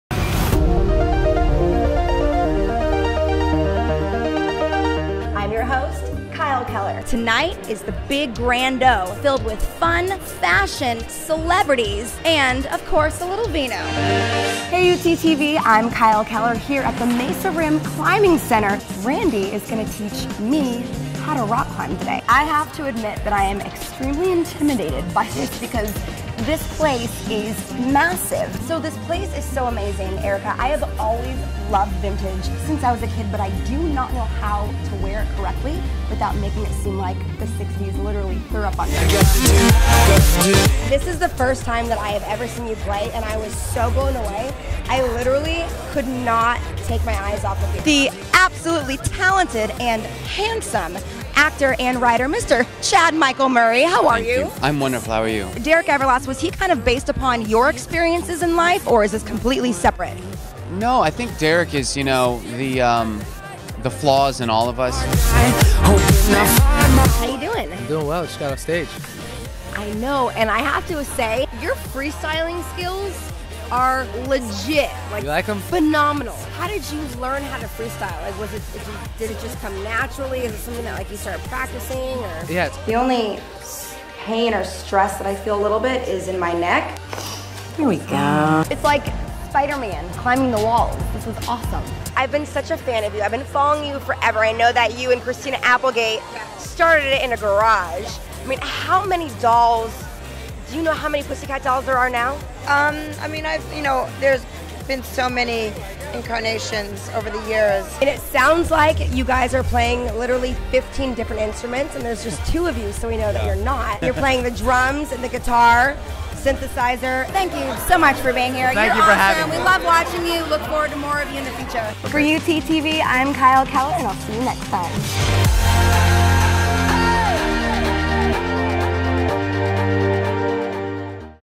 Kyle_Keller_Hosting_Reel_
Kyle Keller's hosting reel for agents, managers and casting directors